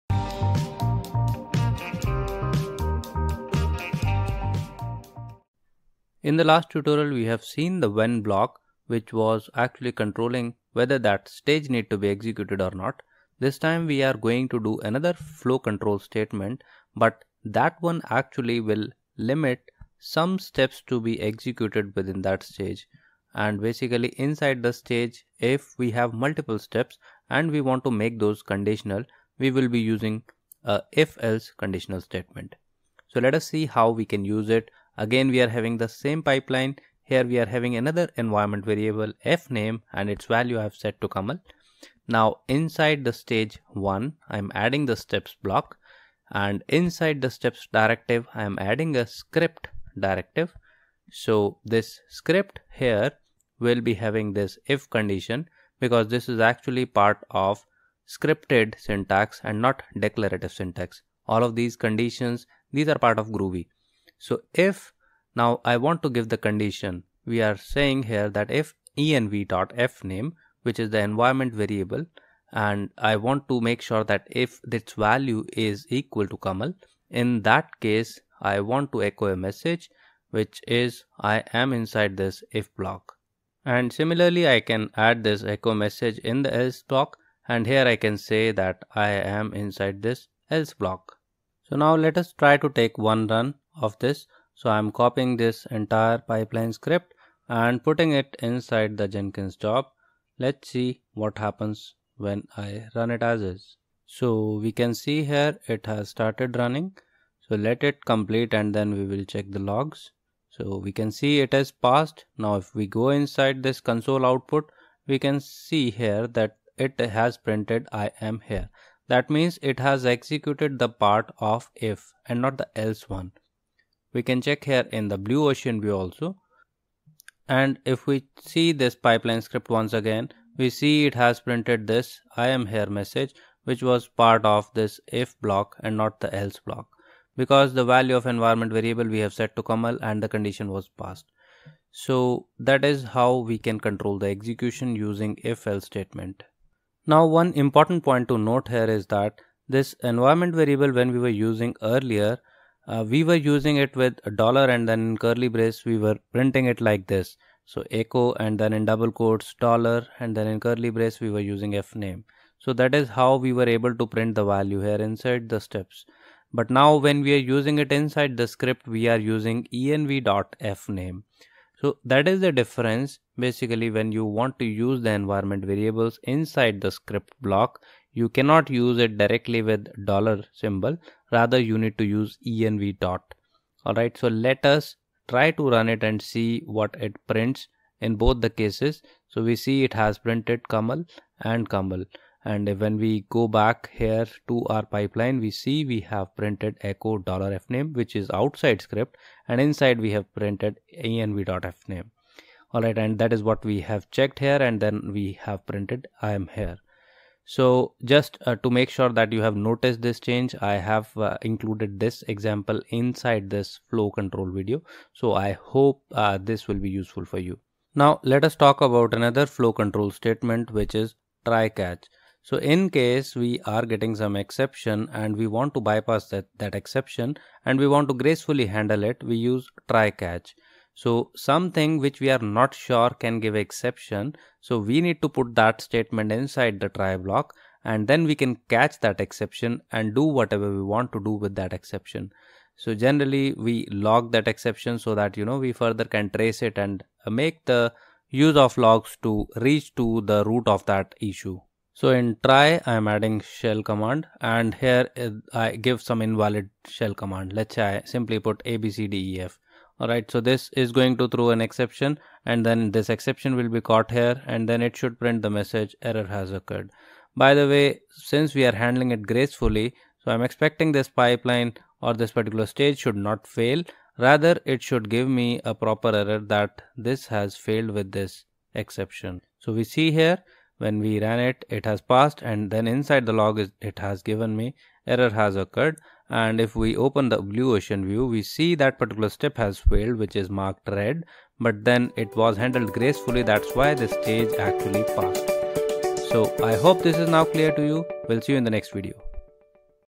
#17 Jenkins Pipelines | Flow Control If Else, Try Catch
In this video, we will cover the Flow Control If Else and Try Catch statements in Jenkins Pipelines. These statements allow you to add conditional logic to your pipeline, making it more flexible and capable of handling errors or exceptions.

We'll cover the syntax for defining If Else and Try Catch statements, including how to specify the conditions and the actions to be taken if the condition is true or false. We'll also demonstrate how to use If Else and Try Catch statements in both scripted and declarative syntax, and how they can be used to handle errors, failures, or exceptions that may occur during pipeline execution.

Additionally, we'll discuss best practices for using If Else and Try Catch statements, including how to structure your pipeline to optimize its performance and make it more readable and maintainable.

By the end of this video, you'll have a good understanding of how to use If Else and Try Catch statements in your Jenkins Pipelines to add conditional logic and handle errors or exceptions, making your pipeline more flexible and reliable.